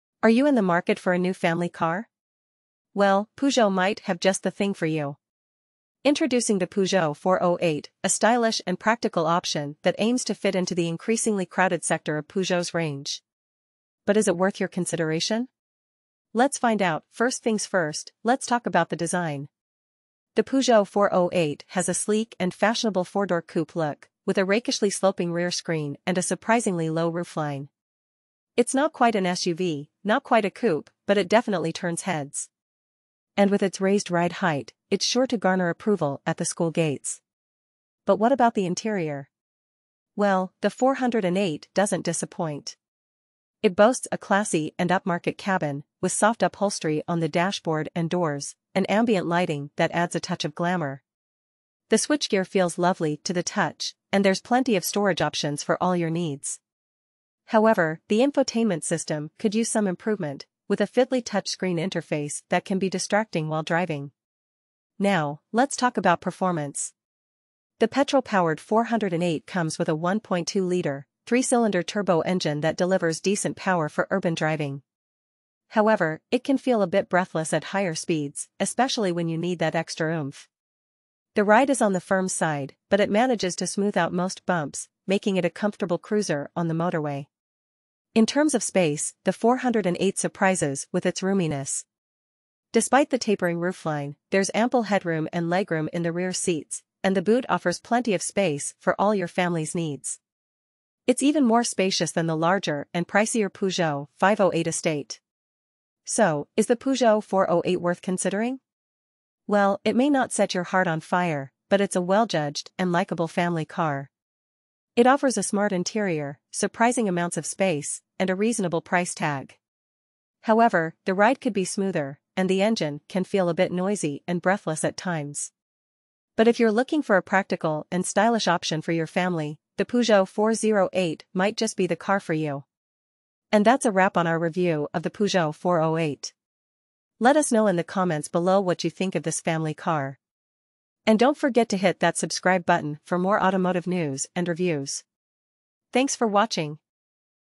Peugeot 408 review: It will garner approval at the school gates, but is ultimately a sensible family
The Peugeot 408 is a family car that combines SUV and four-door coupé styling. It has a fashionable interior with ample space and a smart design. The infotainment system, however, is fiddly and the ride could be smoother. The petrol-powered 408 in Allure Premium form offers a good specification and is reasonably priced compared to its competitors. The car has a distinctive three-cylinder engine that can feel gutless and laggy at times. The ride is firm but acceptable, and the car performs well on motorways. It handles corners adequately but lacks communication and has some body control issues. The 408 is well-judged and likeable, although it may not be particularly memorable to drive. Overall, it is a sensible family car with a stylish appearance. The review rates it four out of five stars.